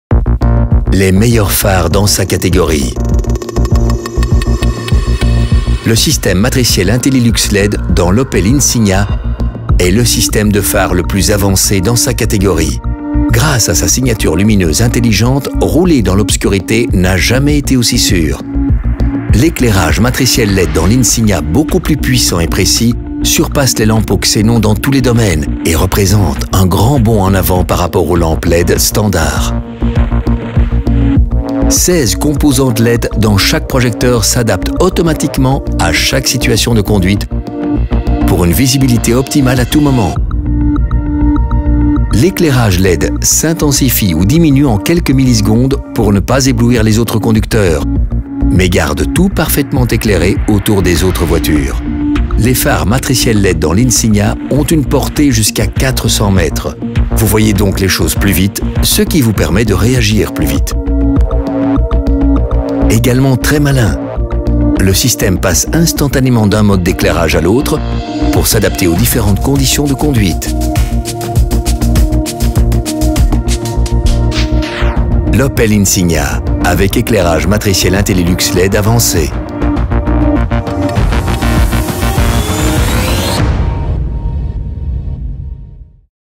Best-In-Class: Matrix LED - la nouvelle Opel Insignia.
IntelliLux LED® Matrix headlights are far superior to Xenon and are a major step up from conventional LED lights –
more powerful and more precise.

Driving with the best light! 32 single LED elements, automatically adapt to different driving situations. Within milliseconds the beam dips around other vehicles
while keeping the rest of the road brightly lit. With up to 400 metres reach, the system makes driving at night safer and more relaxed.